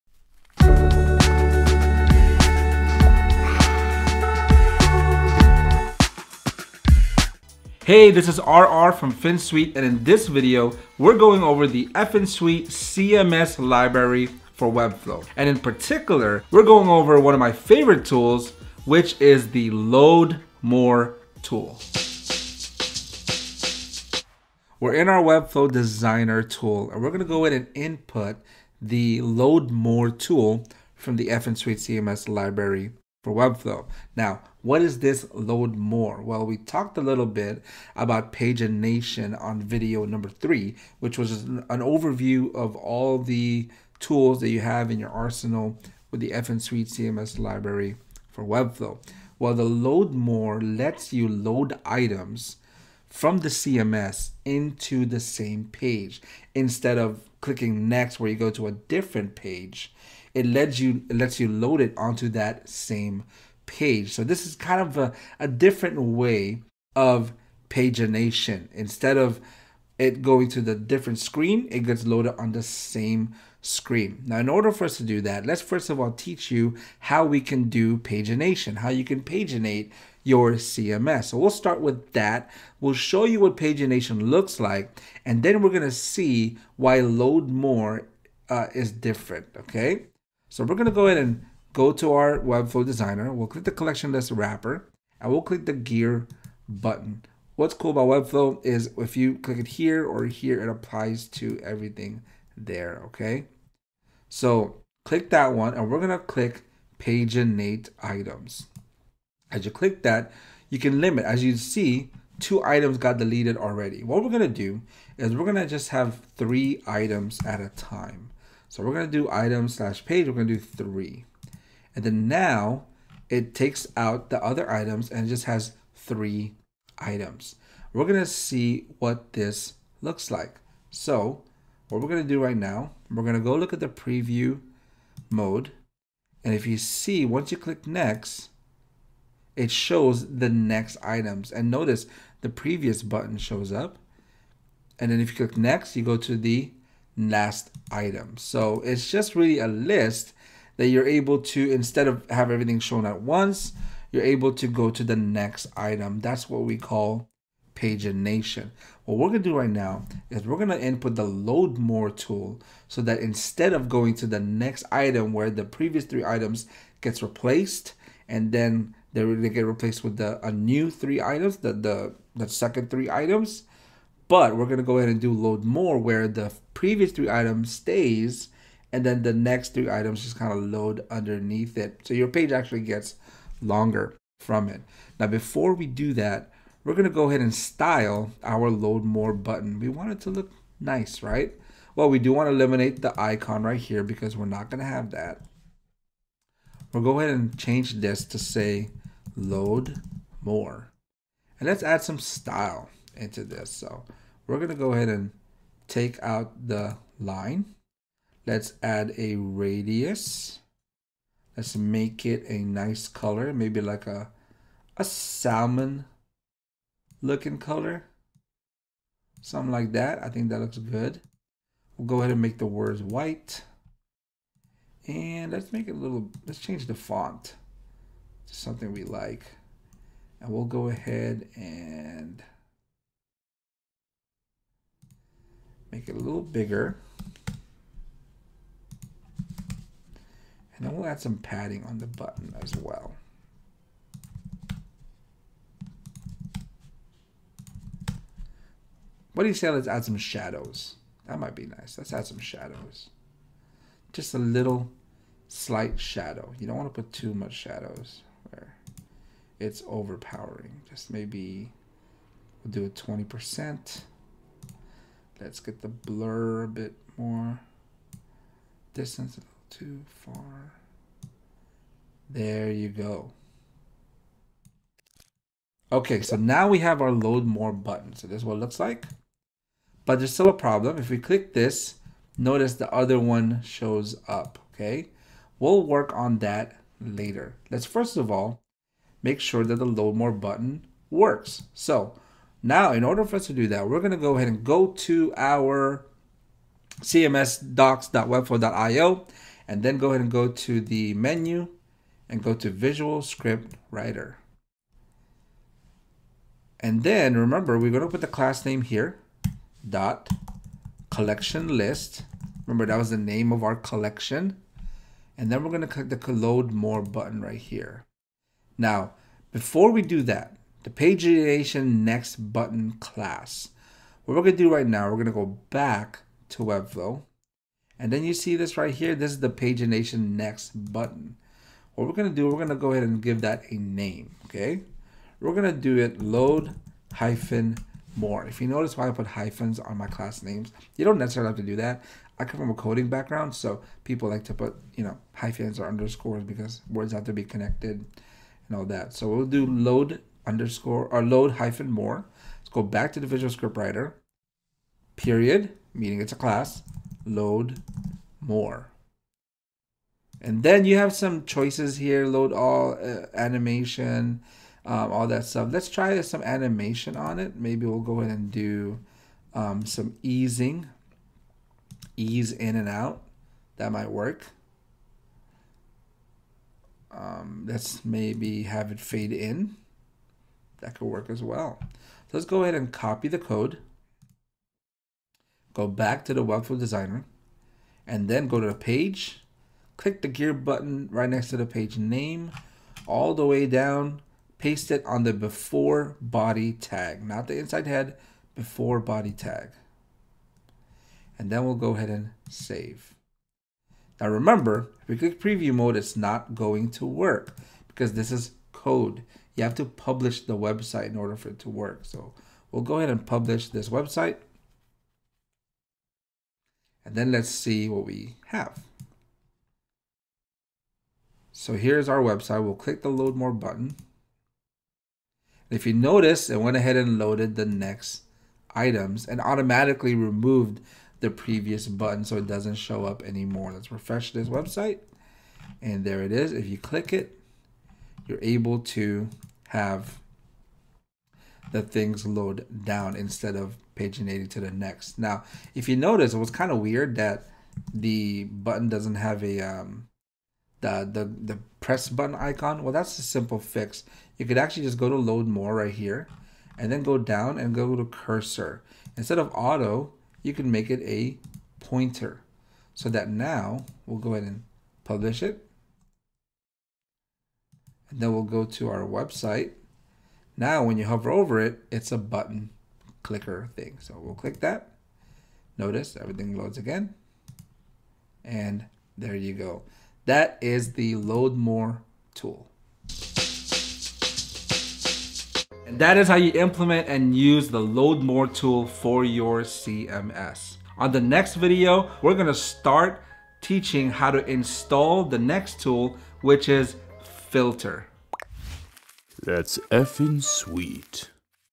(2020) Load More | BeginneRR Series for Finsweet CMS Library
In lesson #6, we learn how to use the Load More component. ⚠️ This system is happily no longer supported. CMS Library is now Attributes. 👇

TIMESTAMPS:
00:06 - Intro
00:22 - What is Load More?
03:04 - Styling the Load More button
04:52 - Generate code with Visual Script Writer
06:44 - Live Example
08:05 - What's next?

// EXPLORE ATTRIBUTES BY FINSWEET

CMS Library is now a legacy solution.

// ABOUT US

// SOCIAL